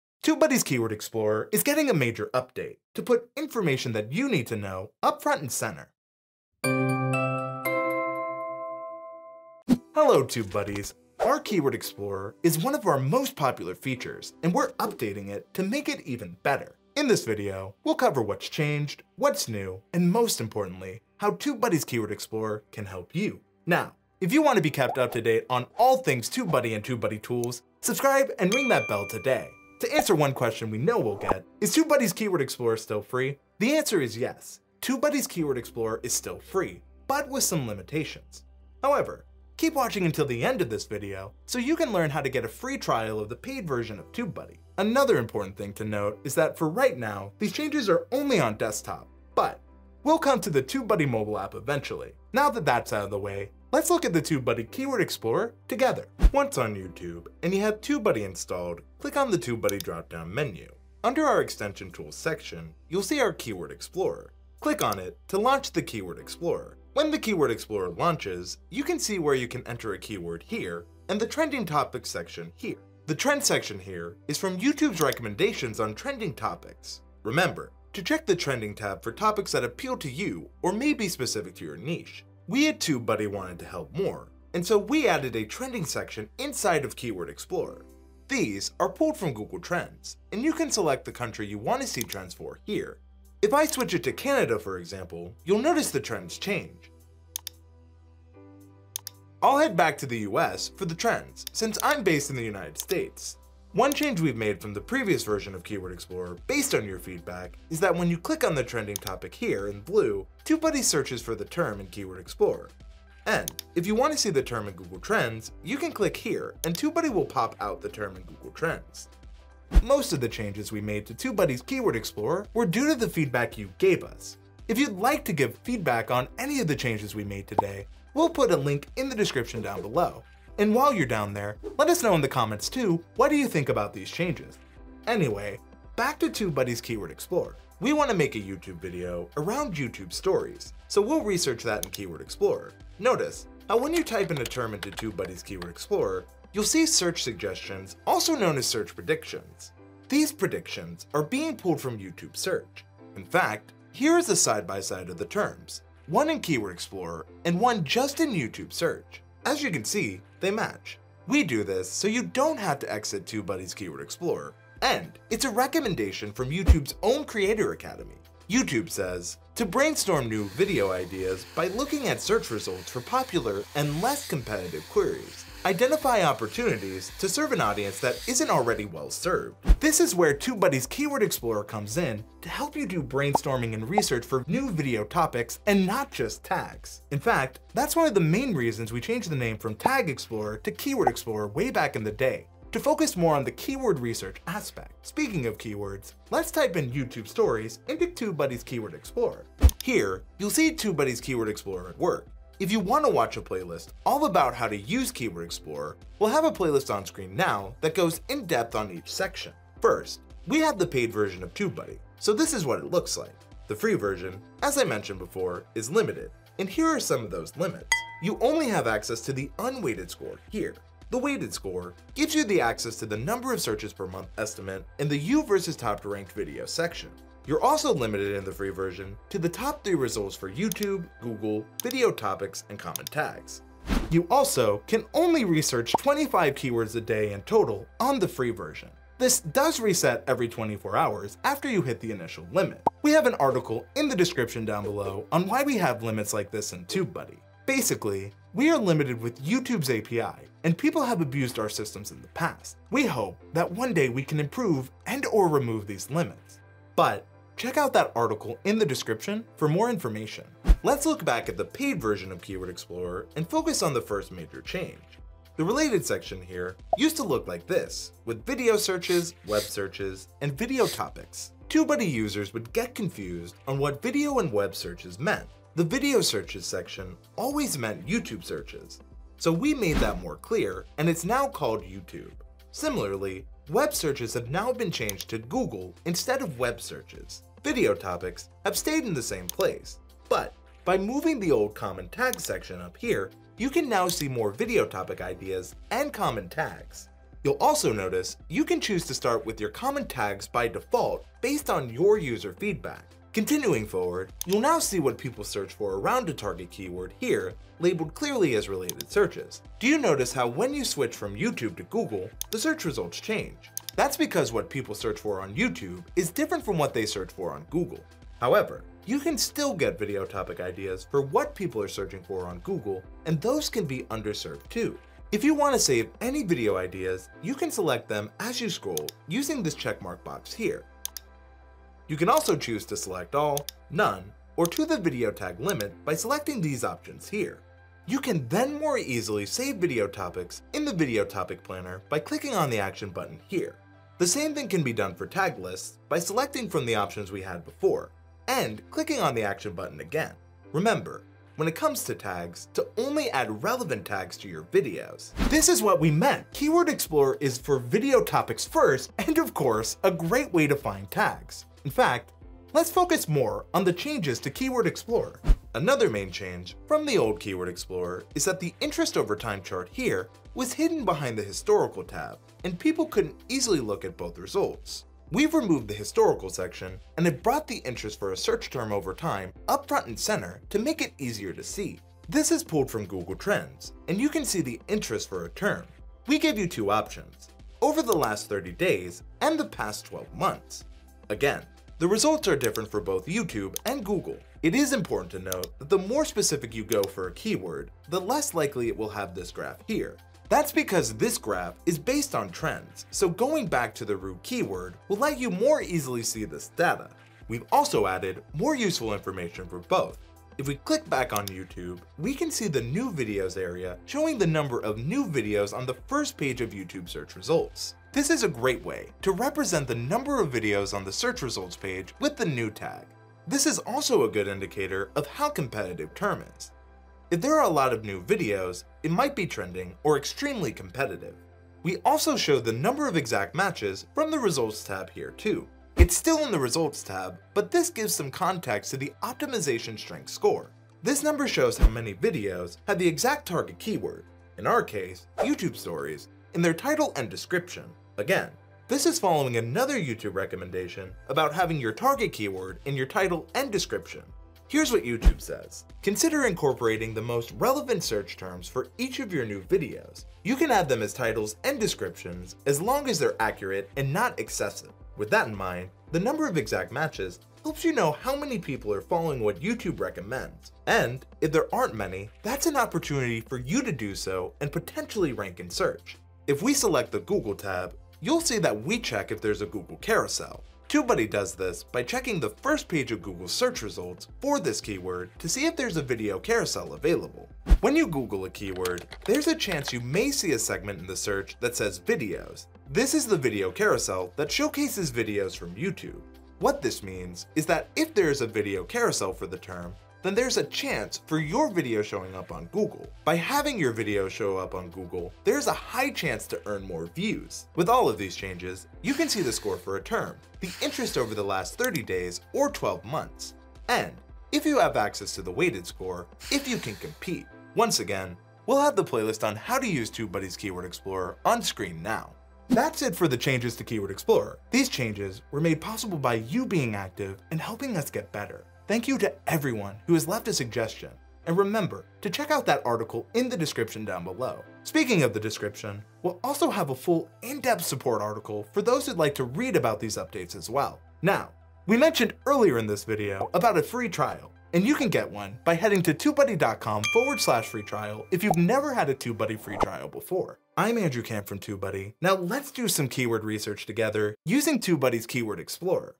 TubeBuddy Keyword Explorer MAJOR update - Keyword Research got easier!
In this video we discuss the TubeBuddy Keyword Explorer MAJOR update - Keyword Research got easier! TubeBuddy's Keyword Explorer is getting a major update. This TubeBuddy Keyword Explorer Tutorial will discuss what those updates are! TubeBuddy's Keyword Explorer is used by millions of users, and we want to thank YOU for helping us make it better!

📖 YouTube Chapters📖
00:00 - TubeBuddy Keyword Explorer Changes - EVERYTHING YOU NEED TO KNOW!
00:30 - Is the TubeBuddy Keyword Explorer still FREE?
00:44 - These Updates are not yet on the TubeBuddy Mobile app!
00:53 - Looking at Trending Topics in the TubeBuddy Keyword Explorer!
01:52 - Doing keyword research with the TubeBuddy Keyword Explorer!
03:19 - What are the limits with the FREE version of the TubeBuddy Keyword Explorer?
04:20 - What’s changed in the TubeBuddy Keyword Explorer?
05:26 - Adding multiple keywords to the Video Topic Planner or a Tag List!
06:07 - What is the graph in the TubeBuddy Keyword Explorer?
06:53 - What does the New Videos number under the YouTube section mean?
07:11 - What does the # Exact Matches under the YouTube section mean?
07:57 - What does a “Vid Carousel” under the Google Section mean?
08:28 - What can I see at a quick glance in the TubeBuddy Keyword Explorer?
08:42 - THANK YOU for suggesting a LOT of these changes
09:01 - Want to read about ALL the UPDATES? Check out the article in the description!
09:10 - Visit to get a TubeBuddy free trial if you haven’t had one before!

Directed, Written, and Produced By:  @AndrewK
Edited By: Stacy Ashley